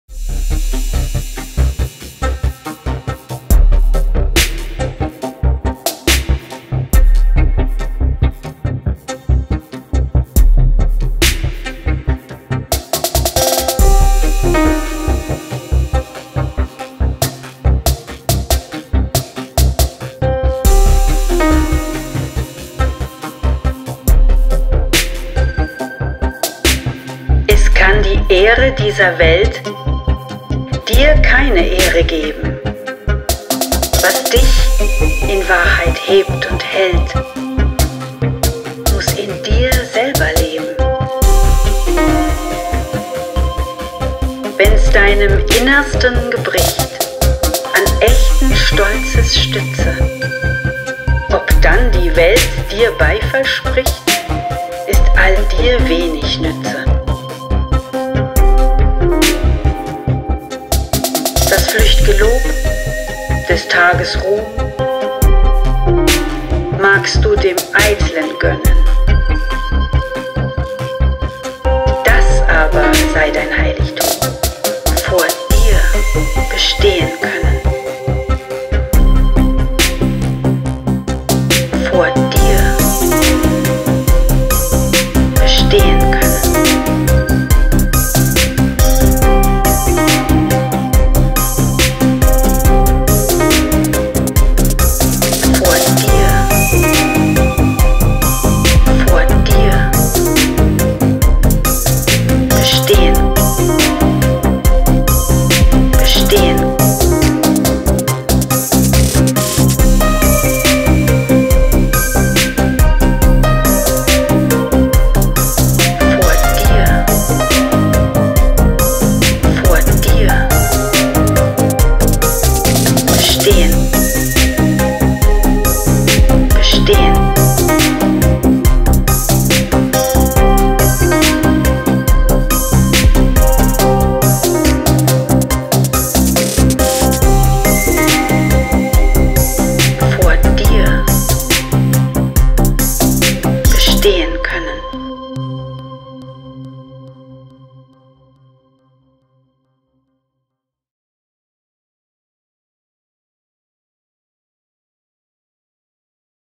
Die Ehre dieser Welt Theodor Fontane
Heinrich Theodor Fontane (* 30. Dezember 1819 in Neuruppin; † 20. September 1898 in Berlin) war ein deutscher Schriftsteller, Journalist und Kritiker. Er gilt als bedeutender Vertreter des Realismus.
"Die Ehre dieser Welt" passt so erstaunlich in unsere heutige Zeit.

Es kann die Ehre dieser Welt
Dir keine Ehre geben,
Was dich in Wahrheit hebt und hält,
Muss in dir selber leben.

Wenn's deinem Innersten gebricht
An echten Stolzes Stütze,
Ob dann die Welt dir Beifall spricht,
Ist all dir wenig nütze.

Das flücht'ge Lob, des Tages Ruhm
Magst du dem Eitlen gönnen;
Das aber sei dein Heiligtum:
Vor dir bestehen können.